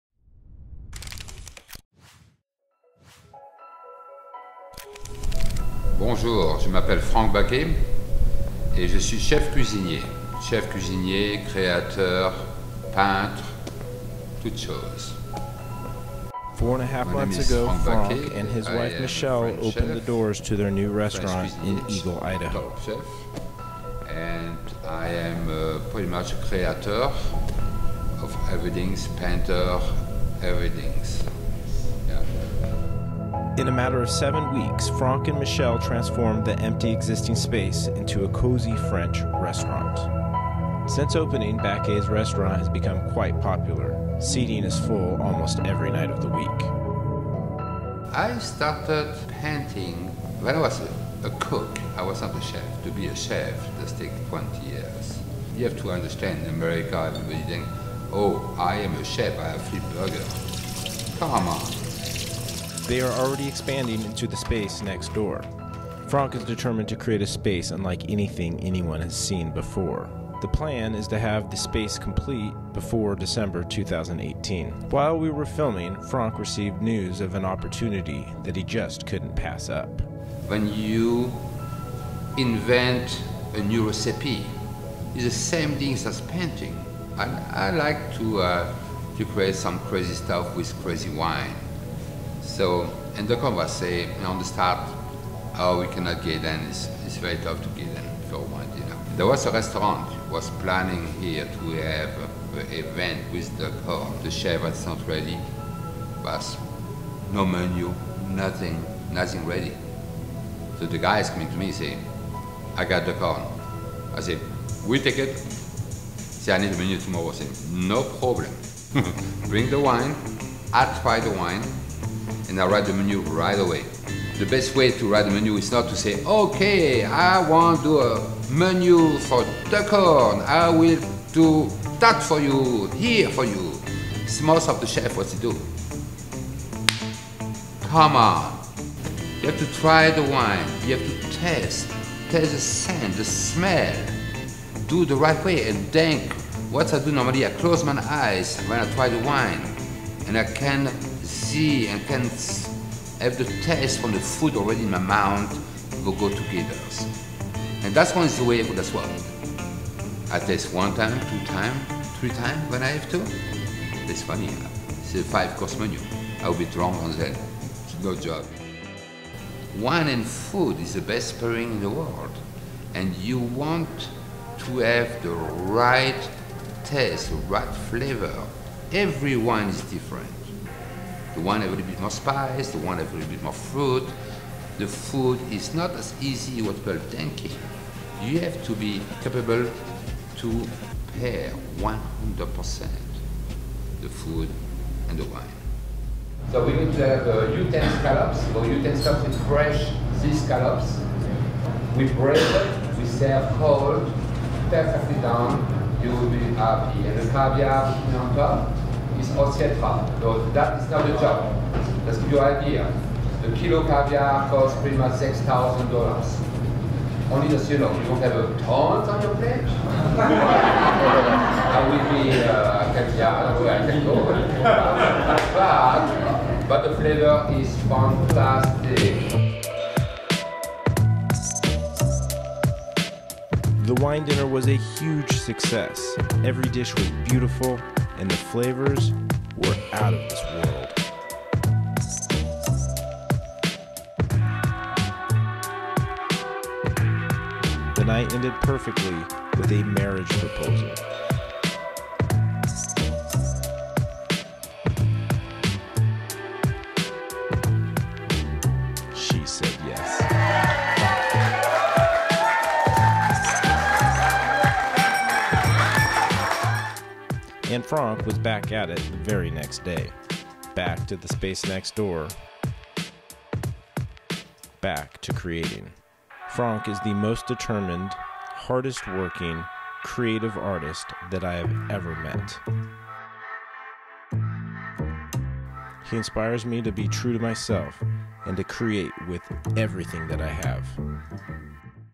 Franck Bacquet - Le Createur
Franck Bacquet is someone that inspires me to be the truest version of myself.  To create with everything I have. This is my  Me and My short film competition.
Franck is a true creator.  A true creative.  He is a master chef, his knowledge and love for wine is legendary.  He doesn't just create beautiful and delicious food, he creates in all mediums.

During the filming of this 5 minute documentary, Franck received news that Duckhorn Winery was looking for a restaurant to host a wine dinner.  The creative process of inventing a menu fit perfect into our story.